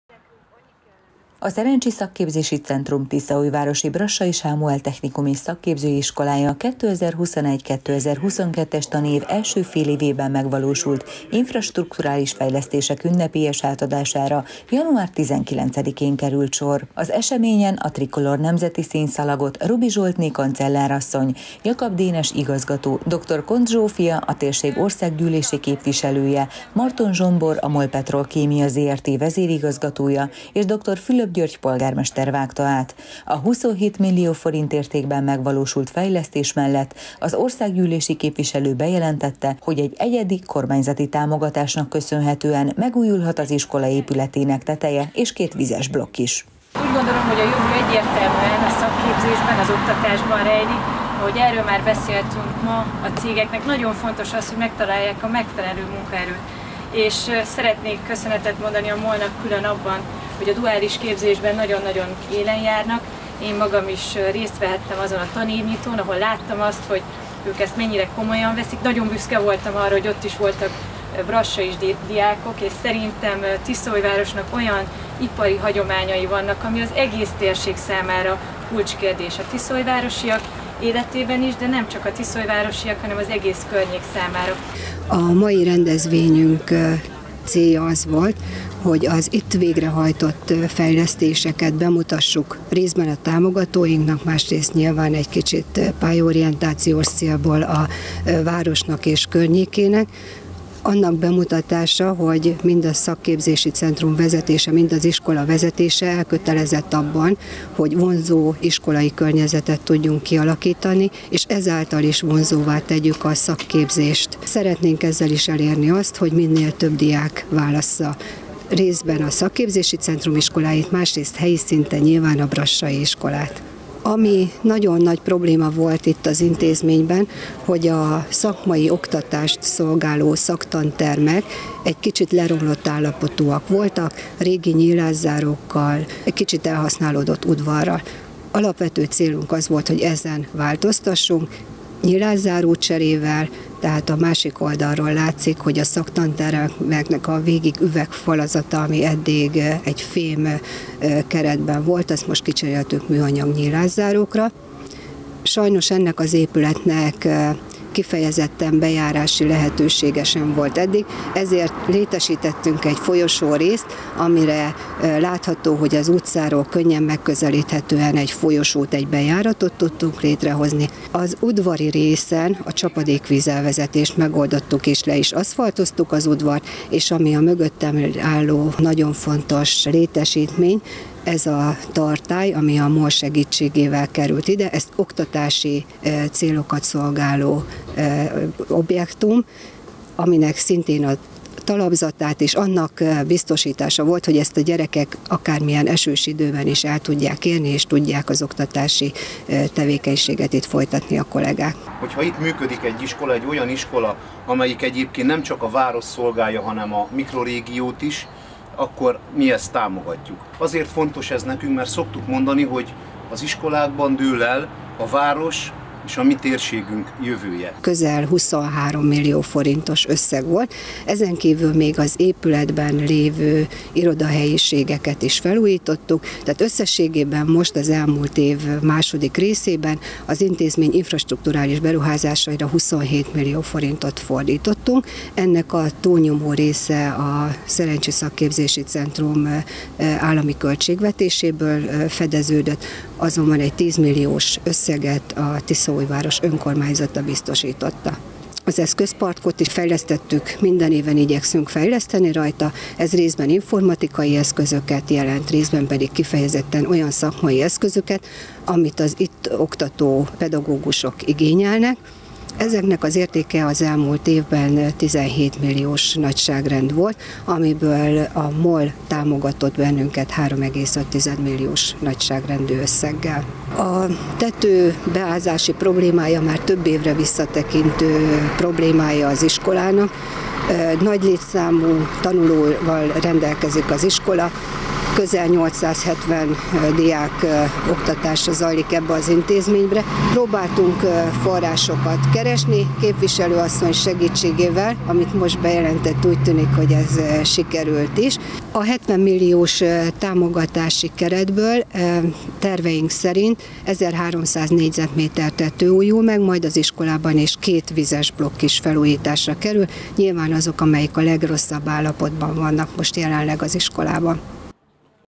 Felújították a tanműhelyet a tiszaújvàrosi Brassaiban
Ünnepélyes keretek között adták át azt az infrastrukturális fejlesztést Tiszaújvárosban, ahol a tanműhelyek nyílászáróit is kicserélték a Szerencsi SZC Tiszaújvárosi Brassai Sámuel Technikum és Szakképző Iskolájàban.